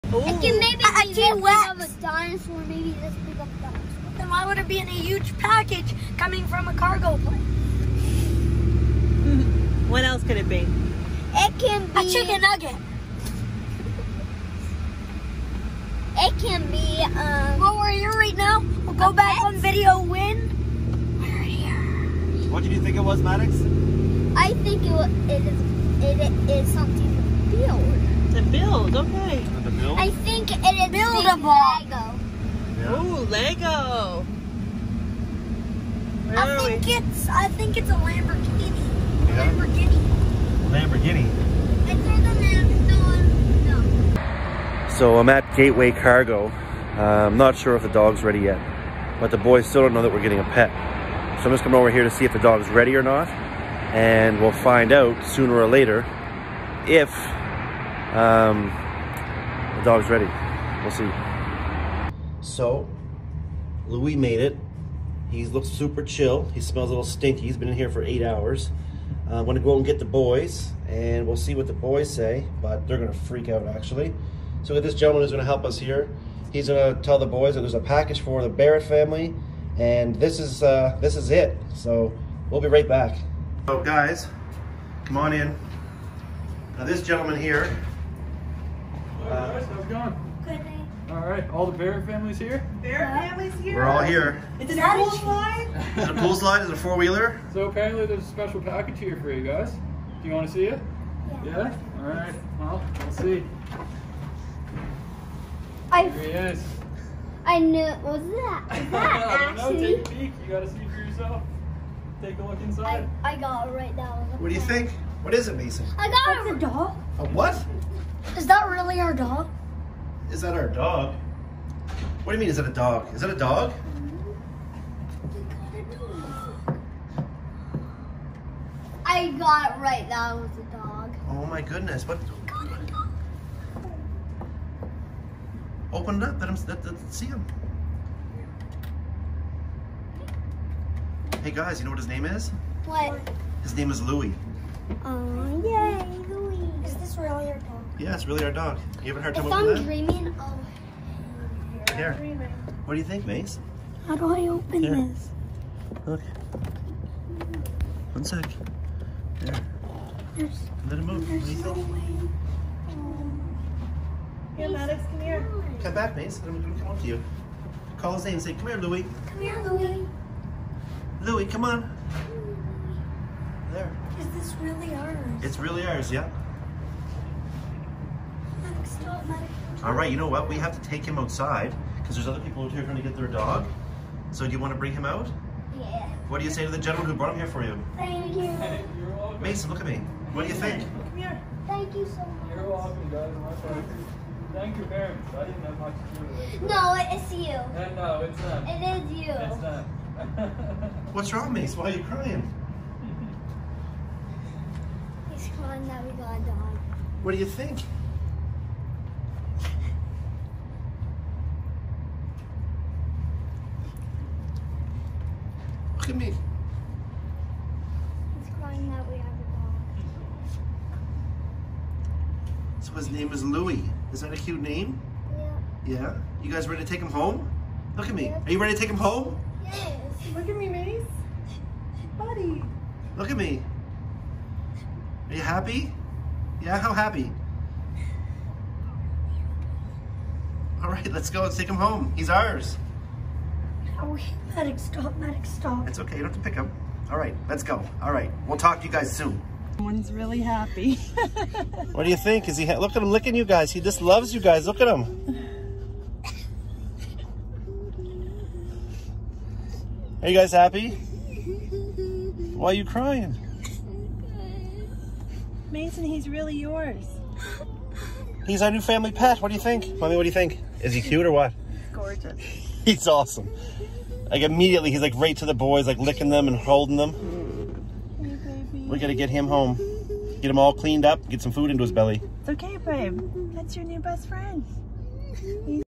St. Berdoodle puppy is a surprise gift to two boys
A St Berdoodle puppy has arrived on a cargo jet as a surprise for two brothers from their parents. The boys had no idea they were getting a puppy but yet a random parcel from the airport. Wait until you see the boys reactions when he sees the puppy for the first time.